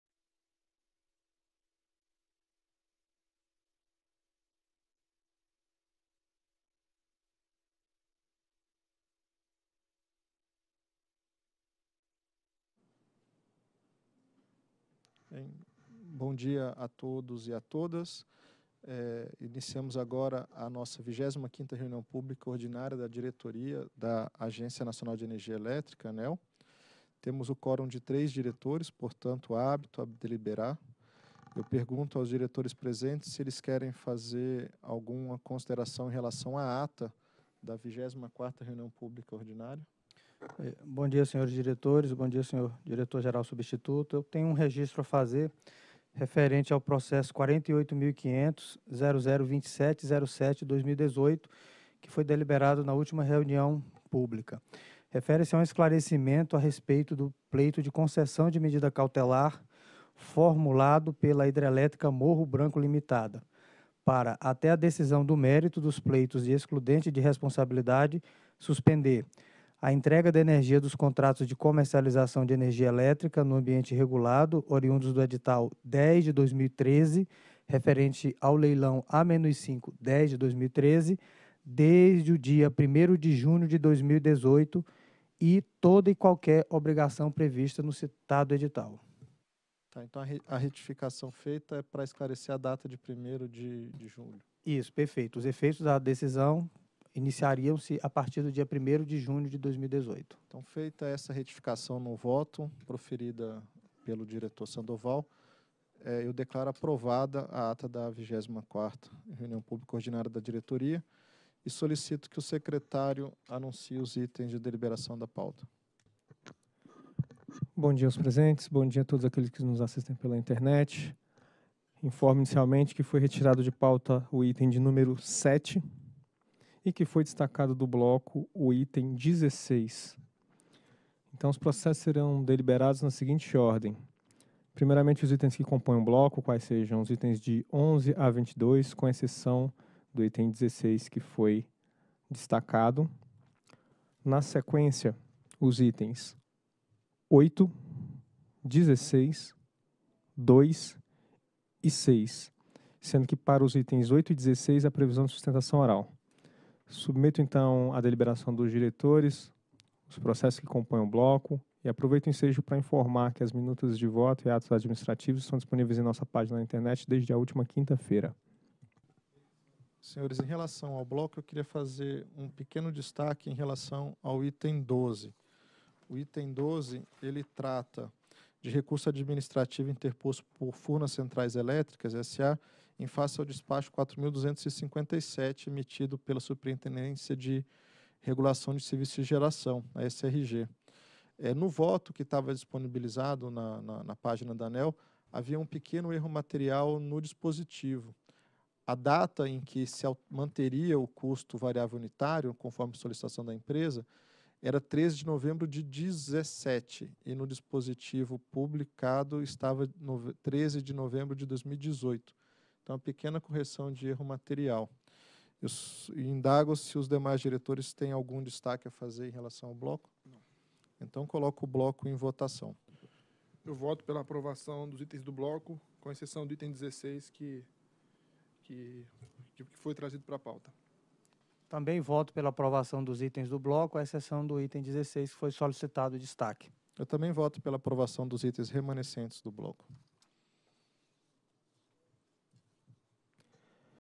Itens 11 a 22 - 25ª Reunião Pública Ordinária ANEEL 17/07/2018
Os itens de 11 a 22 foram deliberados em bloco, conforme o art. 12 da Norma de Organização ANEEL nº 18, revisada pela Resolução Normativa nº 698/2015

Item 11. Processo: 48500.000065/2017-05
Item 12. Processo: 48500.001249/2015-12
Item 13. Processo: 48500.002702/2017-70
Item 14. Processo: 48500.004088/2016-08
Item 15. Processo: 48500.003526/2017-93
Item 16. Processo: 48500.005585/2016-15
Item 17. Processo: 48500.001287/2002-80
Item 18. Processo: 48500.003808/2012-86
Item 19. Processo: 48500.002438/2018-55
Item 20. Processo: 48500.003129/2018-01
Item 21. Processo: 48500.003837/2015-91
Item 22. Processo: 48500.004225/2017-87